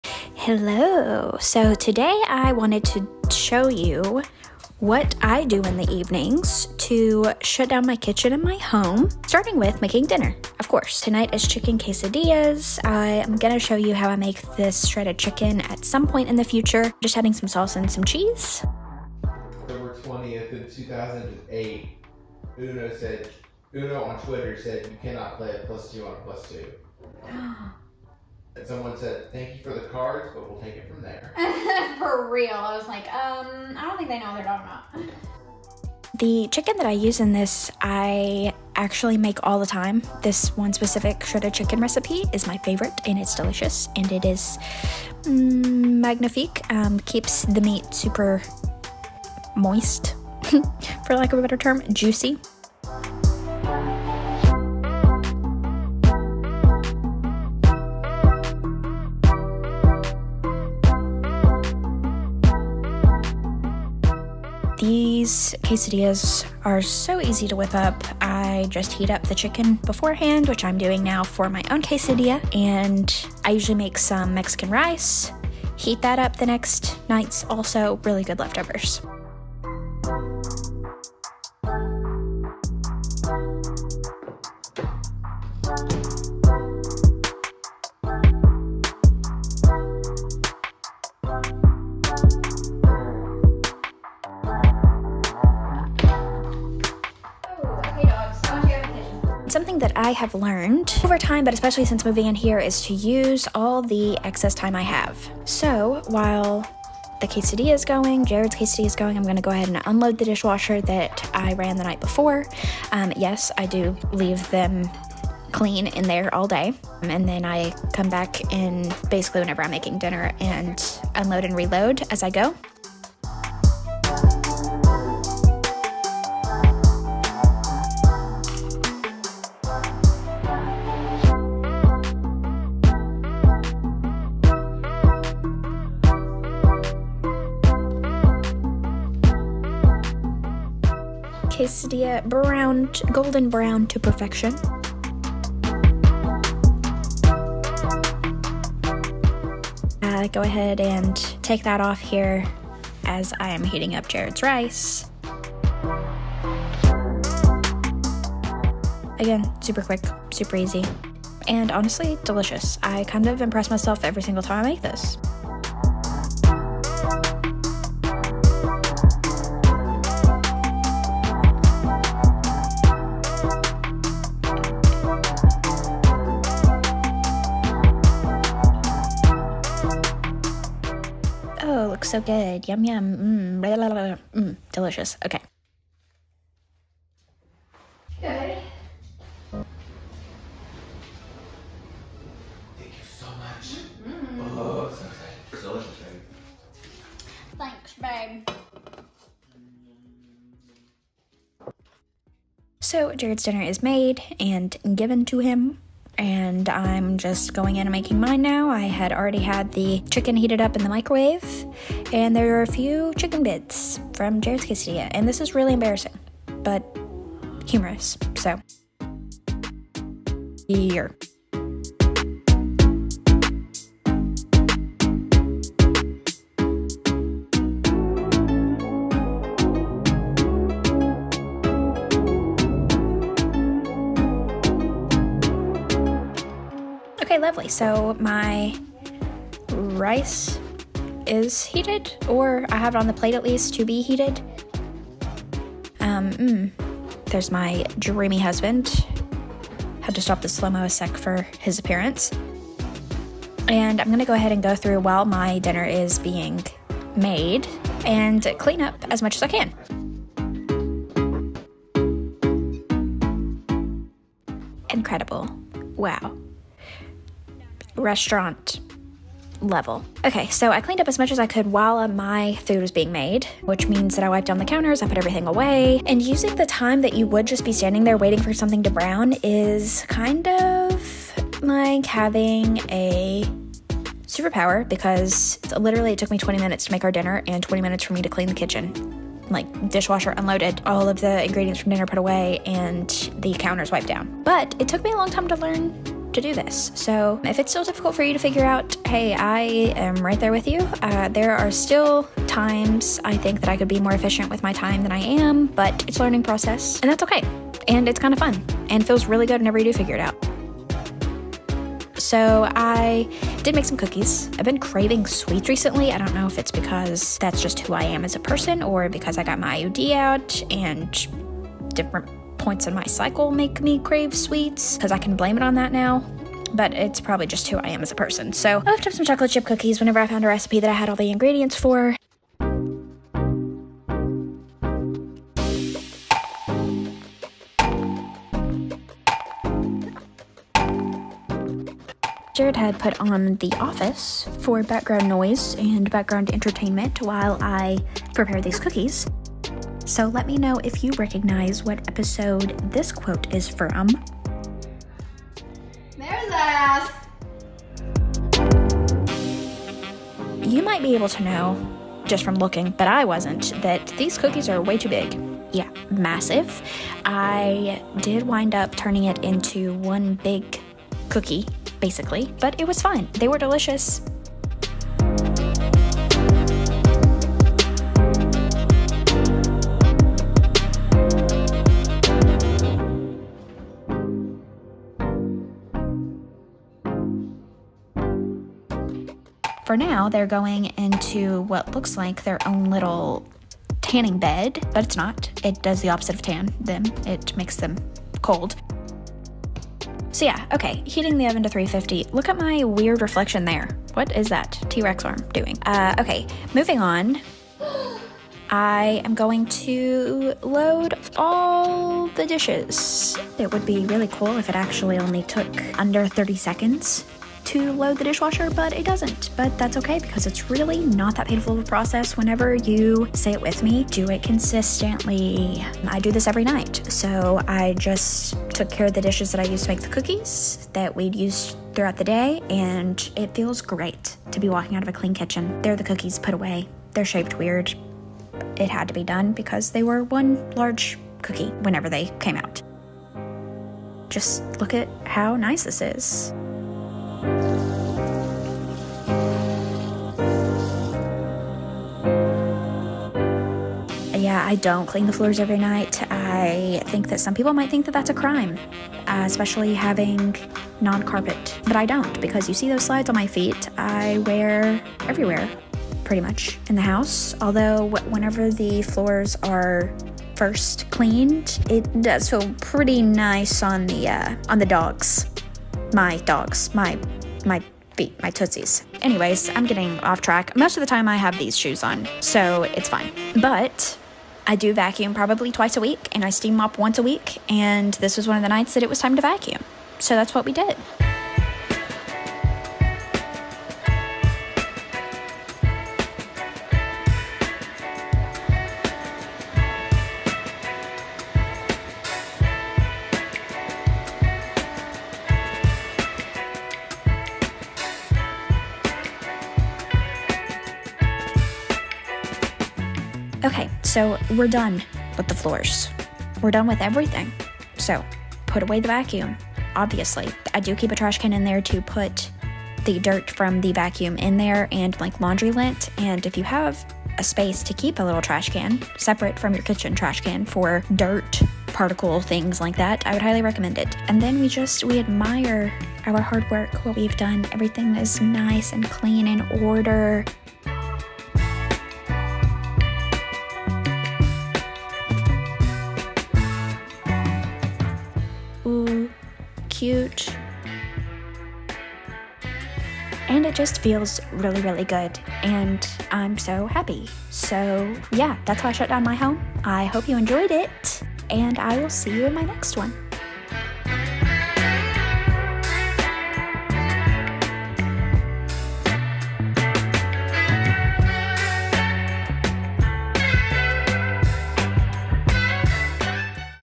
NIGHT ROUTINE 2024: Clean and "Close" My Home with Me!
YEEEELLLO (that's hello in southerner) how ya doin, i hope very well! if you're new here, my husband, Jared, our 2 dogs, Lucy and Micah, and I purchased and moved into our new home February 15th (HELLO, ADULT GOALS? YA, IT'S ME, BEKAH), and have been adjusting very well, albeit still figuring everything out. I've honestly been doing decently well in being a consistent homemaking housewife since we moved in, and here's what I've been doing most evenings to keep up with what needs to be kept up with!